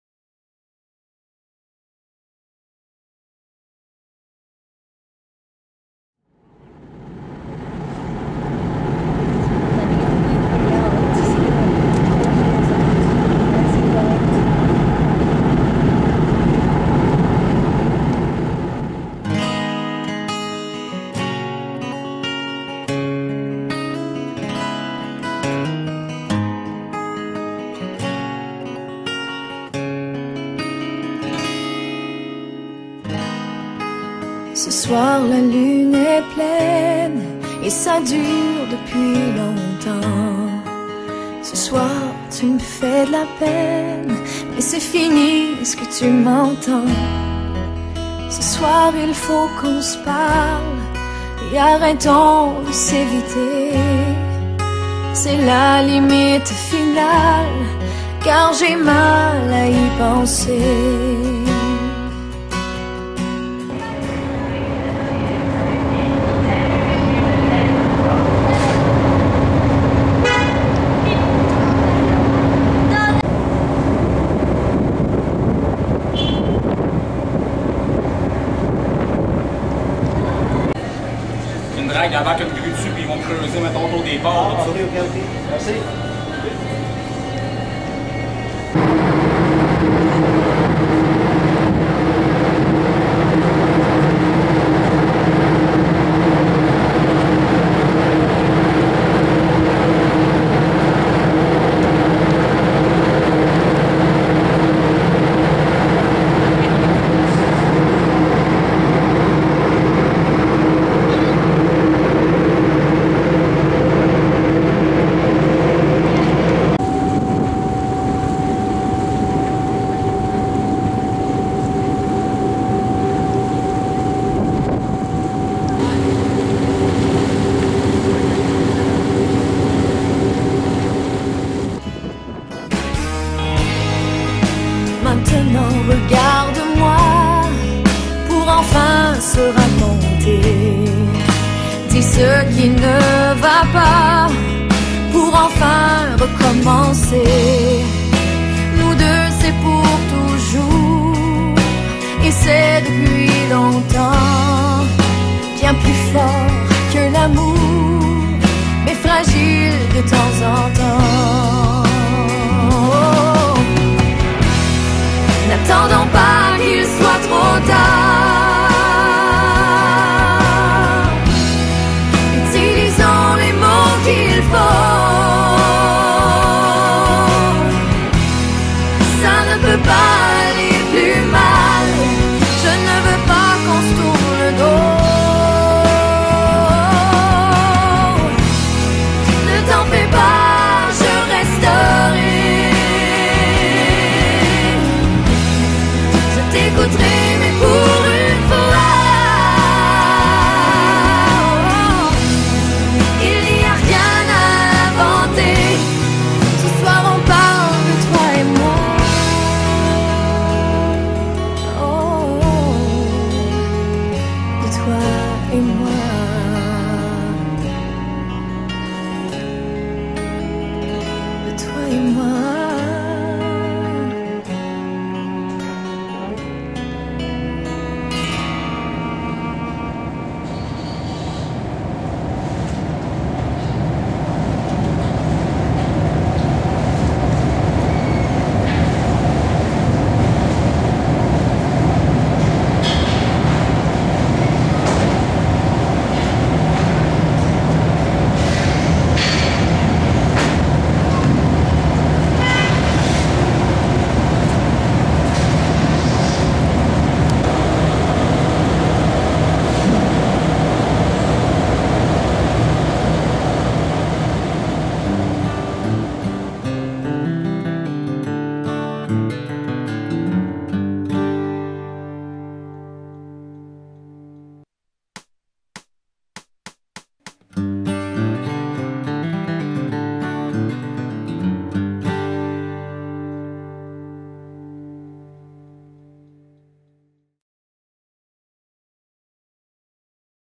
Isla aux Coudres - Canadá
Pasando Qúebec  hacia el noroeste y sobre el Río San Lorenzo se encuentra esta hermosa isla